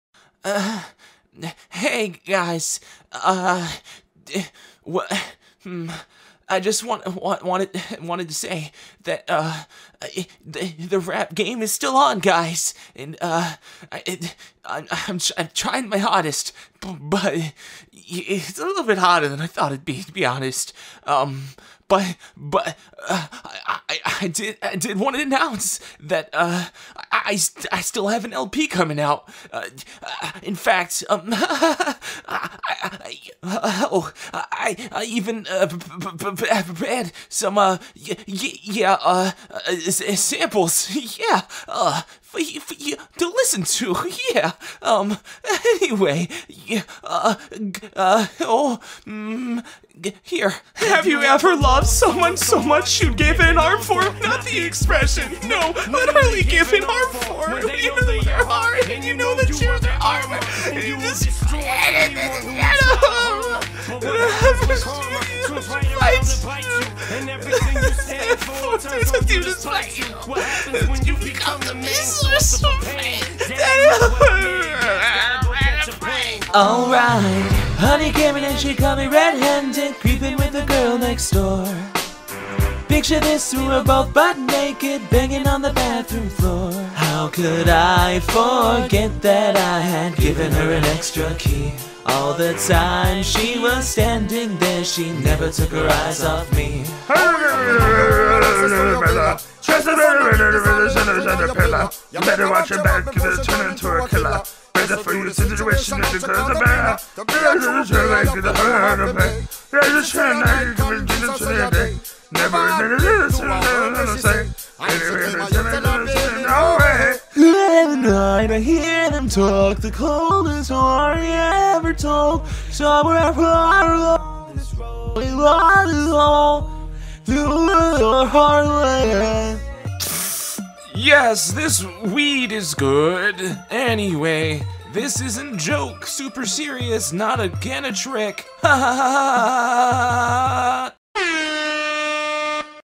Still Quiting
I'm still quitting VA stuff for rap. No, seriously. no. seriously.